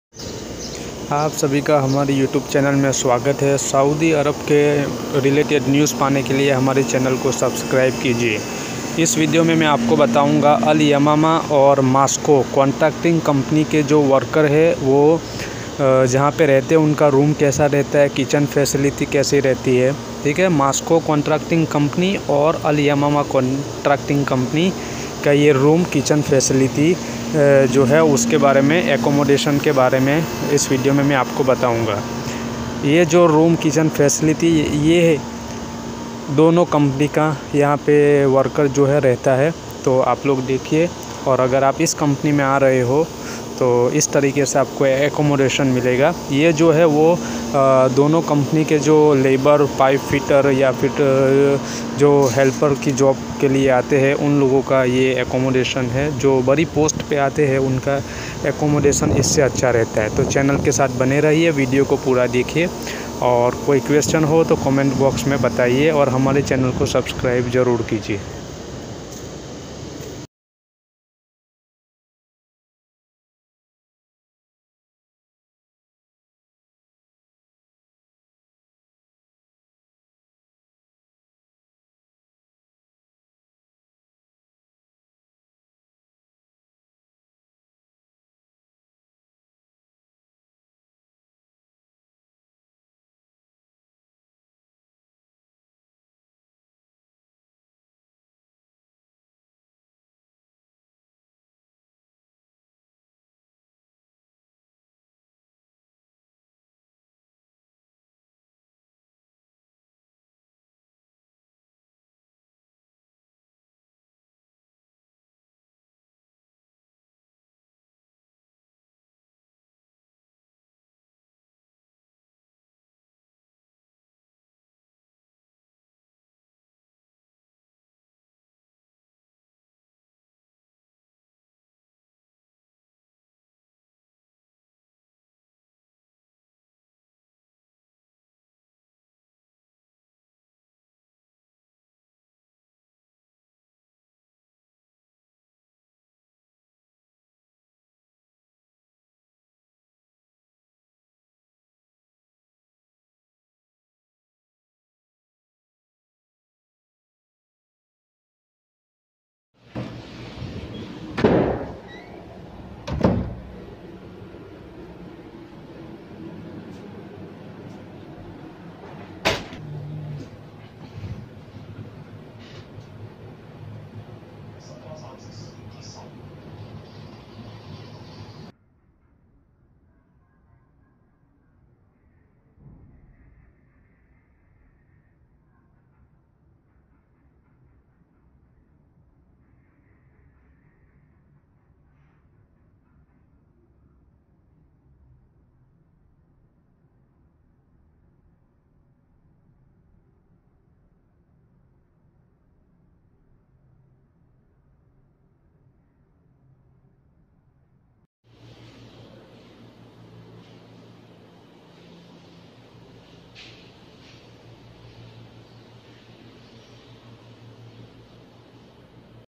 Al Yamama Contracting Company Worker Campus Saudi Arabia Dammam|Accommodation Facility detail
Welcome to our YOUTUBE CHANEL..

Al yamama and masco contracting company saudi Arabia worker campus accommodation details VIDEO.

Subscirbe our chanel for more update